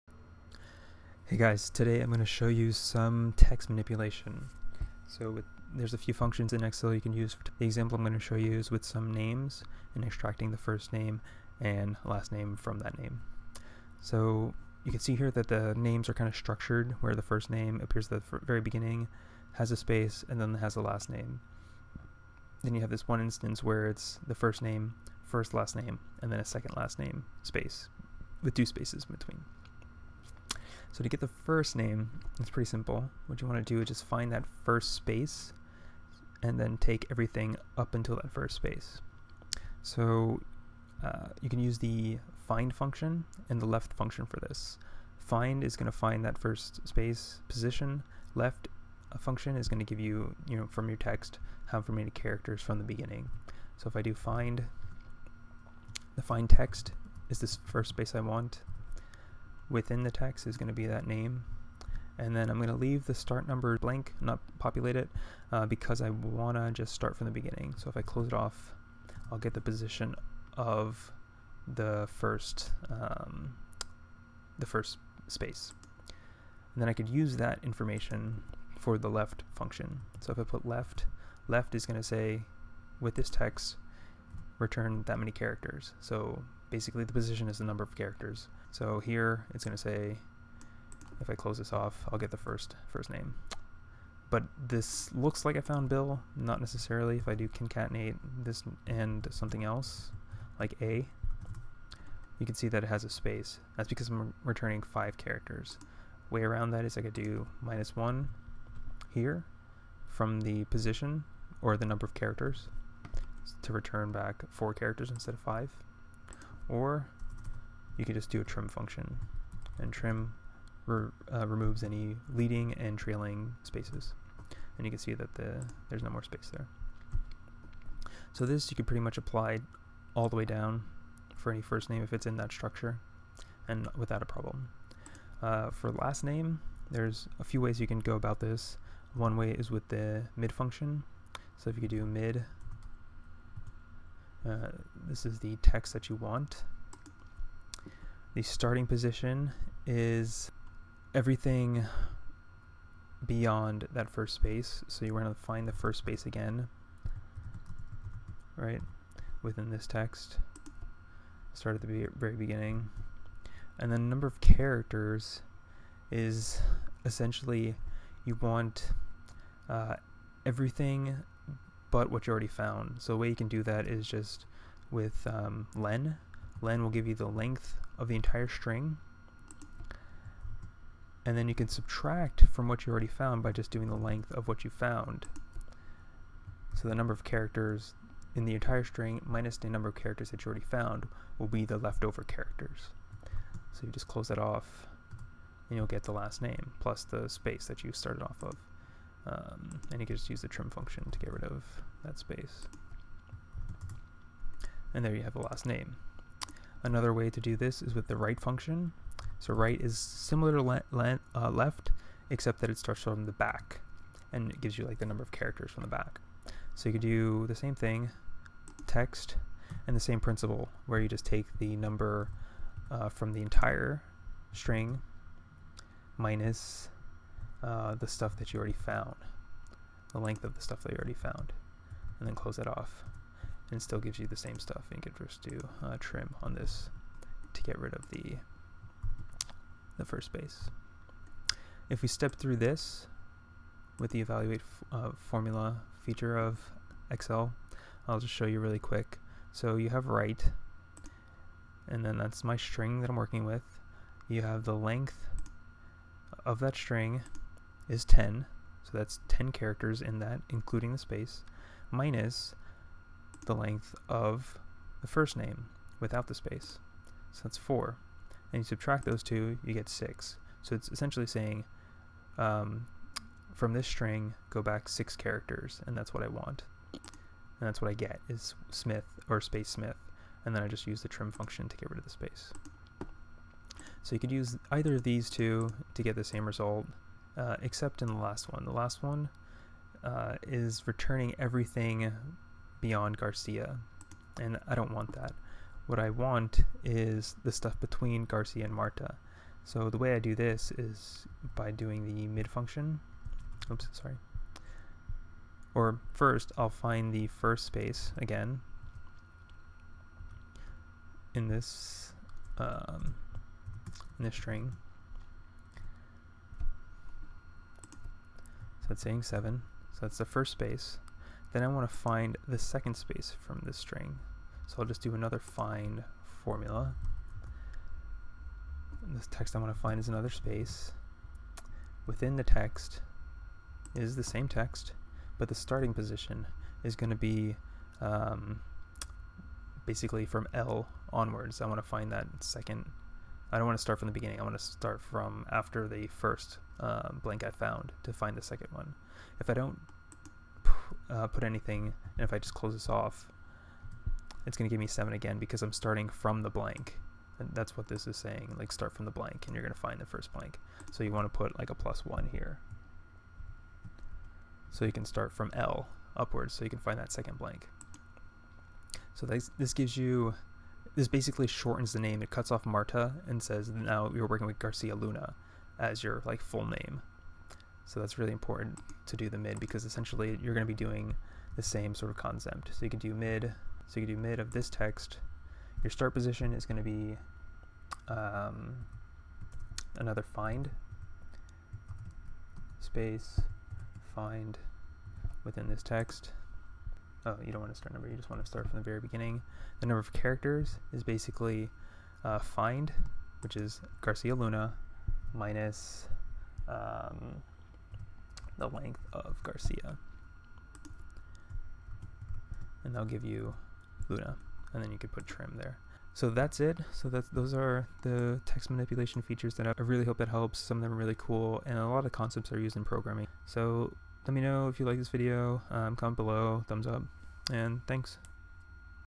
Excel Tips: Extracting Text Between Words and More (MID, LEFT FIND, RIGHT, LEN, TRIM functions)
How to use the LEFT, FIND, TRIM, MID, LEN, and RIGHT functions to extract a word from text

FIND returns the position or the number of characters up to a string you're trying to find

LEFT returns a number of characters from the beginning of the target text

RIGHT returns a number of characters from the end of the target text

TRIM removes leading and trailing spaces

LEN returns how many characters (including spaces) are in the target text

MID returns a number of characters from a starting position

The concept for between text extraction is to get position at the end of the text you don't want, and the count of the entire string minus what you don't want.